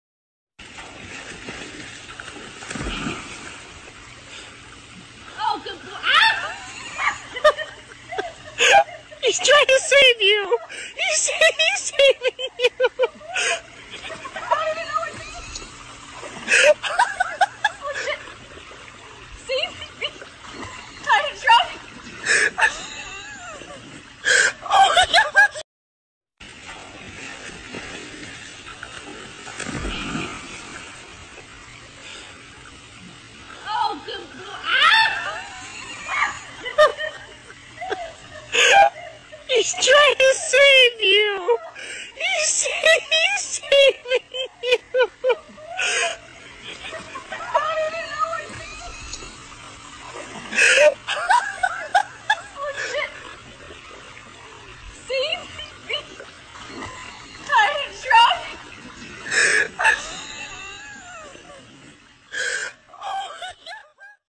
Dog is rescuing a girl||rescue||trained dog||swimming pool
A dog is rescuing a girl, that is very trained dog